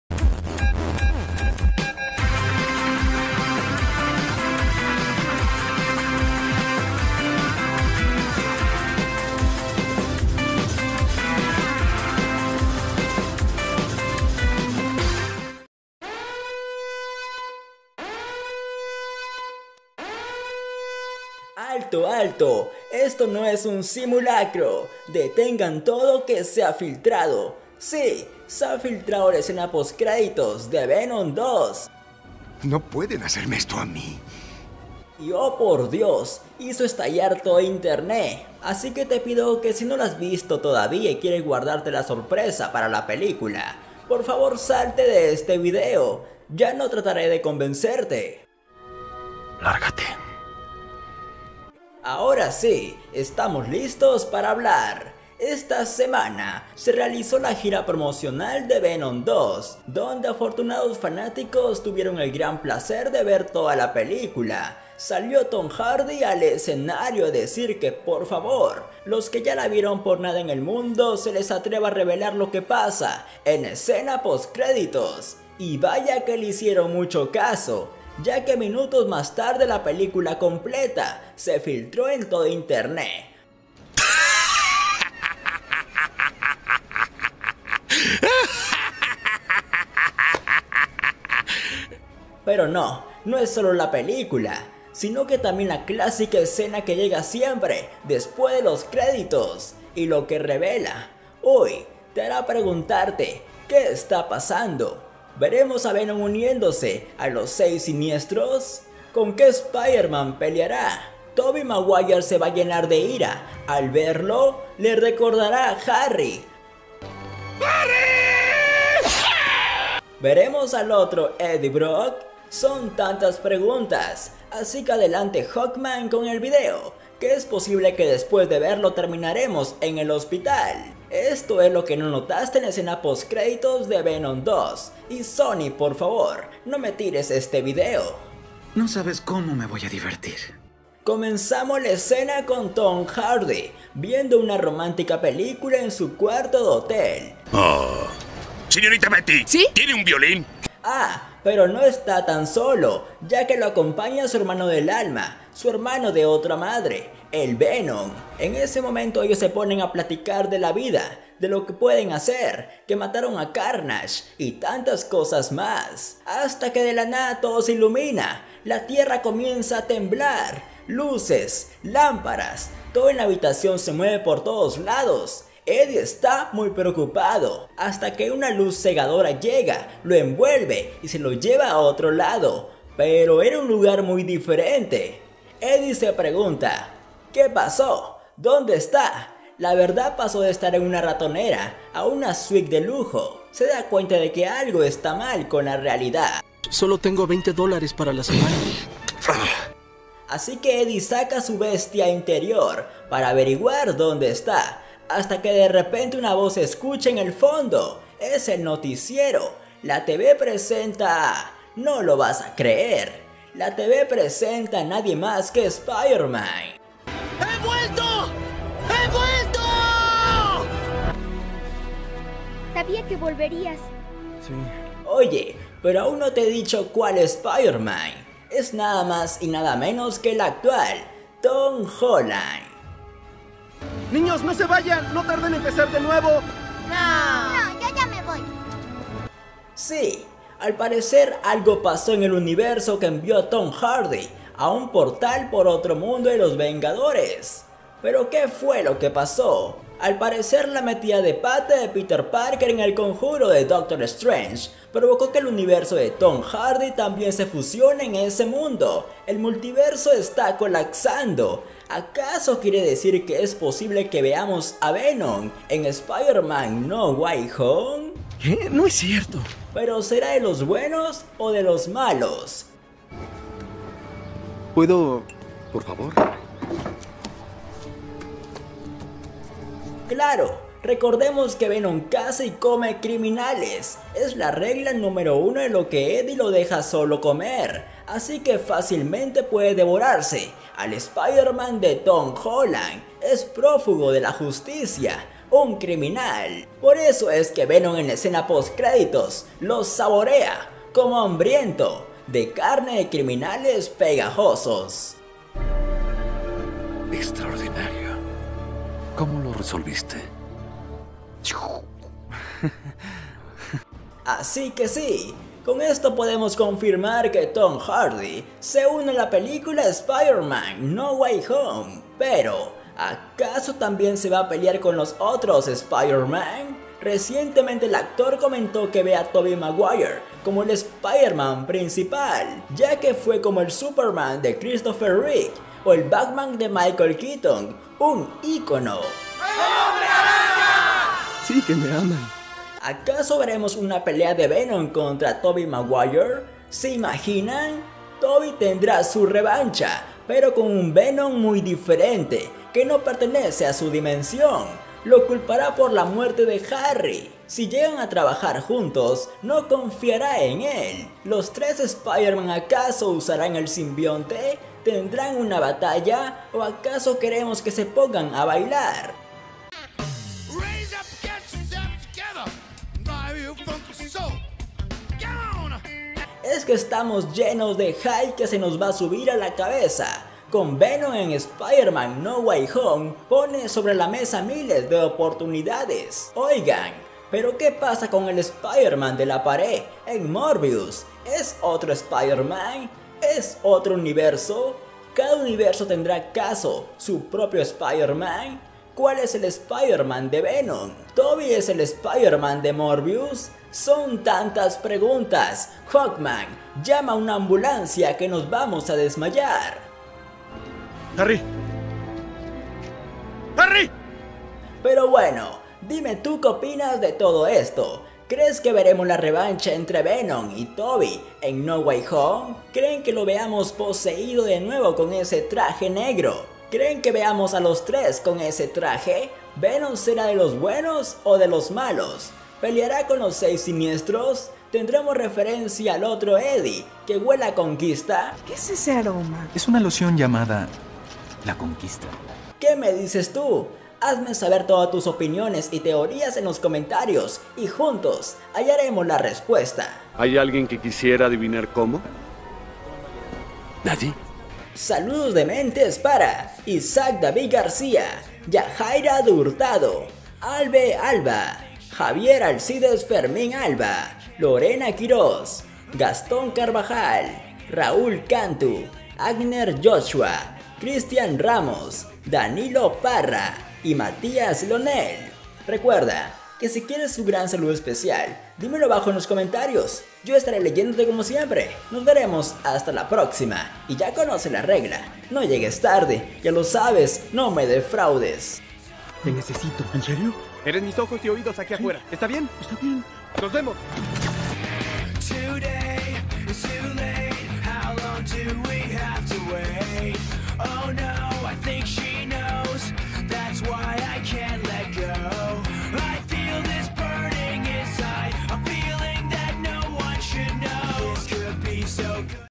¡Imposible! Esto PASARÁ Con VENOM en Spiderman 3 ¡Venganza de Tobey Maguire! Usará Traje Negro?
ALTO! ALTO! ESTO ES NO ES UN SIMULACRO! DETENGAN TODO QUE SE HA FILTRADO! SI SE HA FILTRADO LA ESCENA POSTCREDITOS DE VENOM 2. Y OH POR DÍOS! HIZO ESTALLAR TODO INTERNET. ASÍ QUE TE PIDO QUE SI NO LA HAS VISTO AÚN, Y QUIERES GUARDARTE LA SORPRESA PARA LA PELÍCULA, POR FAVOR SALTE DE ESTE VIDEO, YA NO TRATARÉ DE CONVENCERTE!
Y LO QUE REVELA ¡UY! TE HARÁ PREGUNTARTE! QUÉ ESTA PASANDO. VEREMOS A VENOM UNIENDOSE A LOS SEIS SINIESTROS? CON QUÉ SPIDERMAN PELEARÁ? TOBEY SE LLENARÁ DE IRÁ AL VERLO! LE RECORDARÁ A HARRY! VEREMOS  AL OTRO EDDIE BROCK? SON TANTAS PREGUNTÁS. ASI QUE ADELANTE  HOFFMAAN CON EL VIDEO, QUÉ DESPUÉS DE VERLO, ES SEGURO QUE TERMINAREMOS EN EL HOSPITAL. ESTO ES LO QUE NO NOTASTE, EN LA ESCENA POSTCREDITOS DE VENOM 2. Y SONY POR FAVOR!